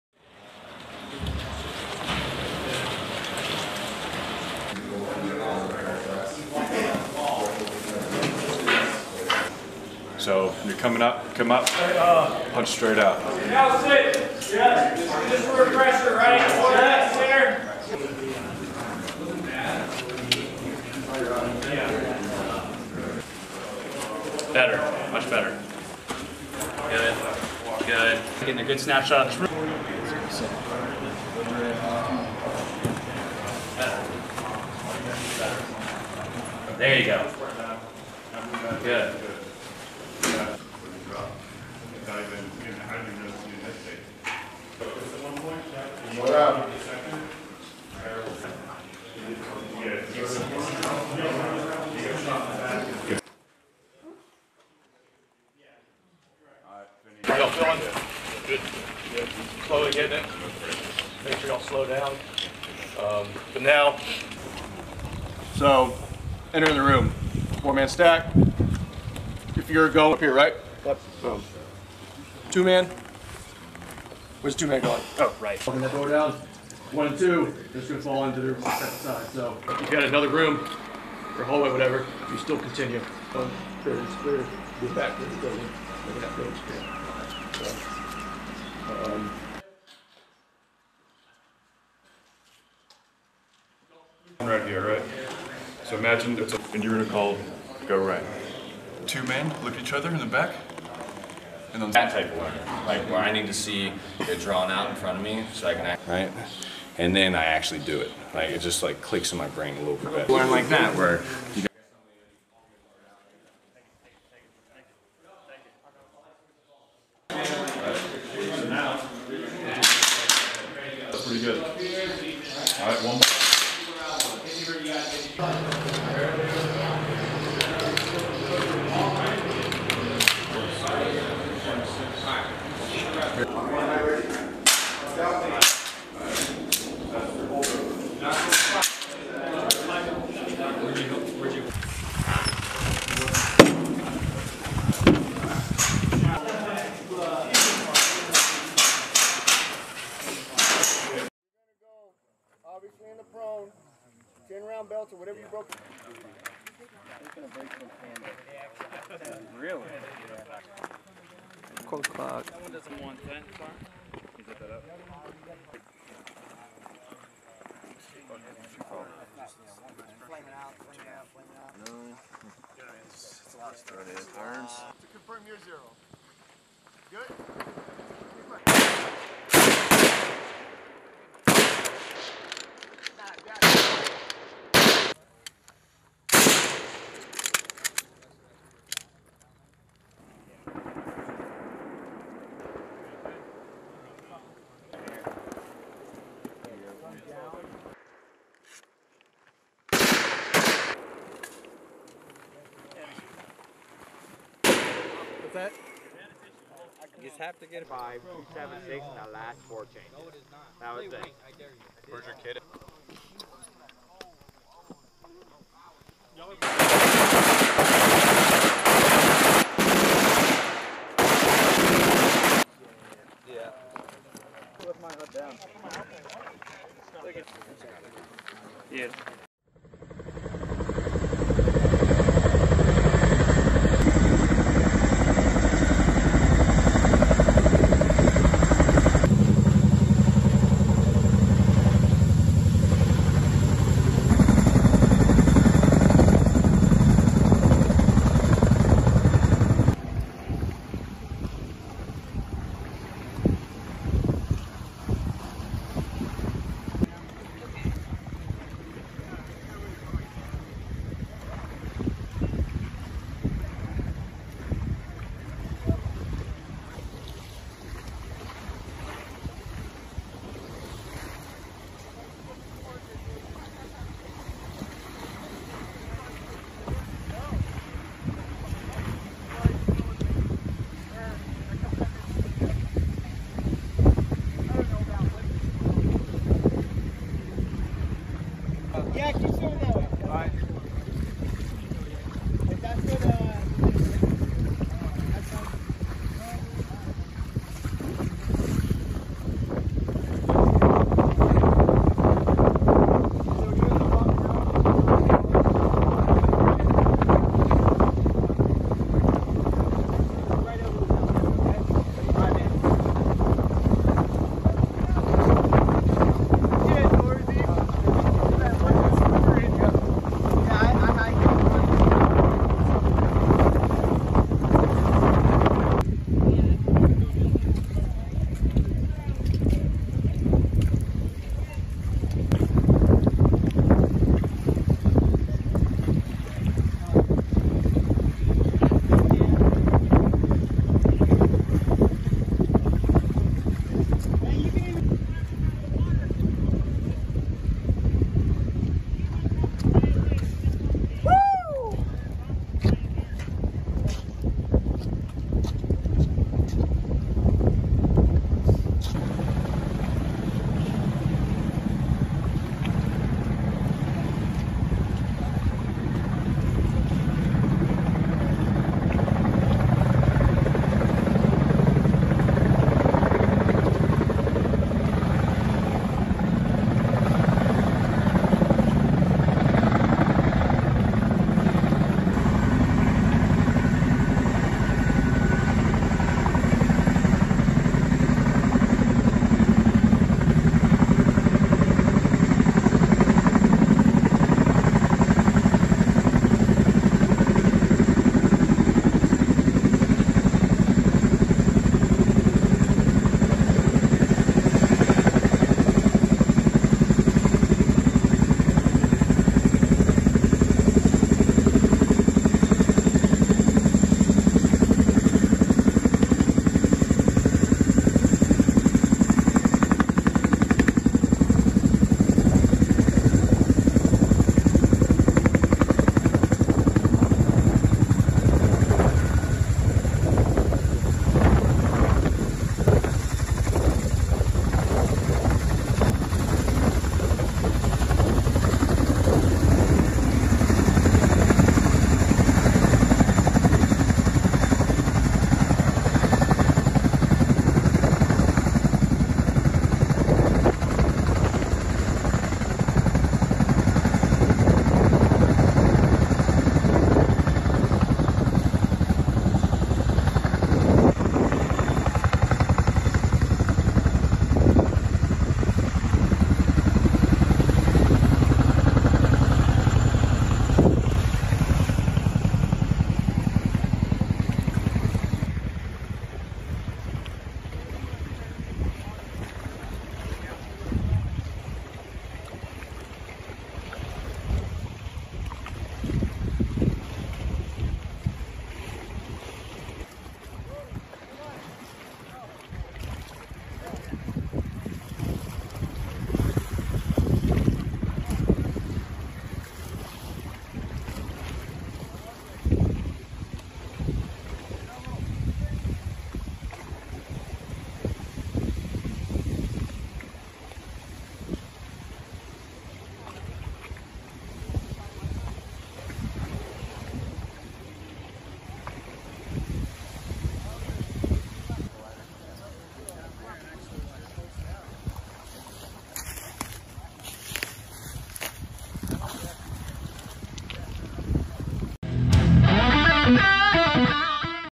U.S. Army. Soldiers of the 10th Mountain Division Test the Visual Augmentation system.
The Integrated Visual Augmentation System (IVAS) is a augmented reality headset being developed by Microsoft for the United States Army, providing a wide variety of new capabilities to soldiers. Originally developed for infantry, it is also being adapted for use by mounted soldiers and aircrew. Its development was initiated in 2018 and is currently undergoing testing.

00:00 Soldiers from 3rd Special Forces Group (Airborne) train Soldiers from 1st Brigade Combat Team, 10th Mountain Division on close quarter battle operations.
02:32 Soldiers Test Integrated Visual Augmentation System.
03:50 10th Mountain Division Combat Aviation Brigade Conducts Training in the St. Lawrence River.